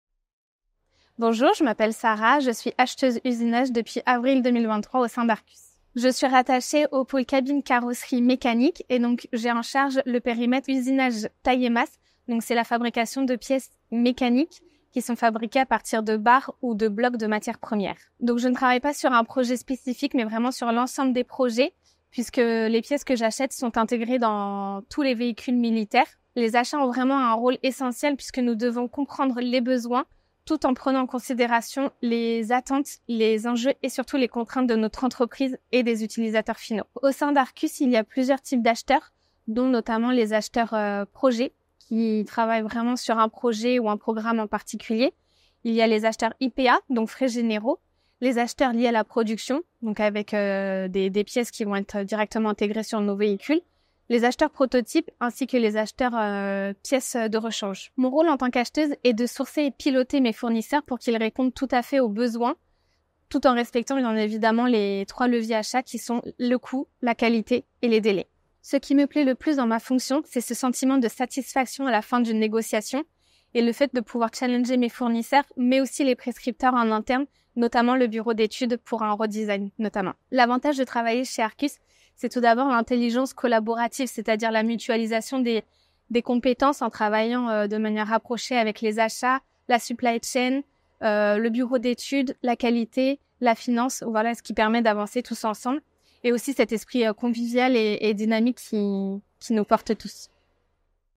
Découvrez Arquus avec Sarah, Acheteuse usinage
[Nos employés ont du talent]

Sarah, acheteuse usinage depuis avril 2023 est rattachée au pôle cabine carrosserie mécanique. Elle est en charge du périmètre usinage taille et masse, impliquant la fabrication de pièces mécaniques à partir de barres ou de blocs de matière première. Ces pièces sont intégrées dans tous les véhicules militaires produits par l'entreprise.

Les achats jouent un rôle essentiel chez Arquus, nécessitant une compréhension approfondie des besoins, des attentes, des enjeux et surtout des contraintes de l'entreprise et des utilisateurs finaux.

Le rôle de Sarah en tant qu'acheteuse consiste à sourcer et piloter ses fournisseurs afin qu'ils répondent aux besoins tout en respectant les trois leviers achats : le coût, la qualité et les délais.